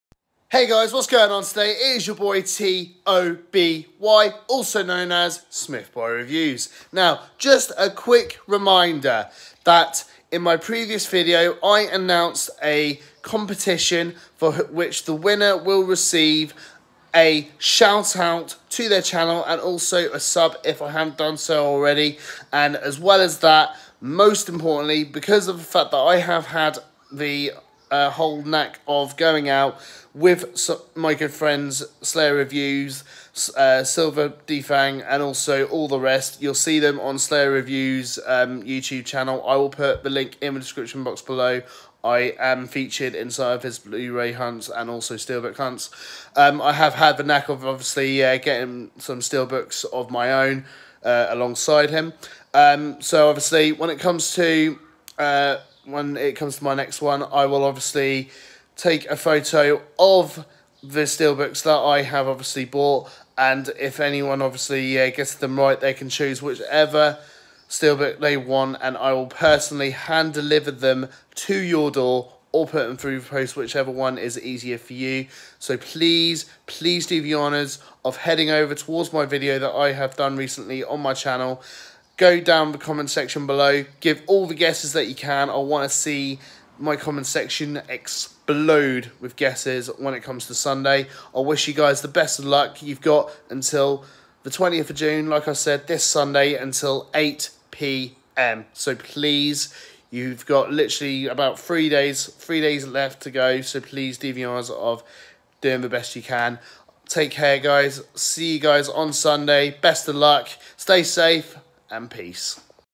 Just a quick reminder….
TIME IS RUNNING OUT GUYS!!! Competition closes at 8pm this Sunday 20th June so head over to my recent video, fill my comments section with guesses of what you think the steelbook I will be unboxing will be!! Best of luck to you all and take care!!!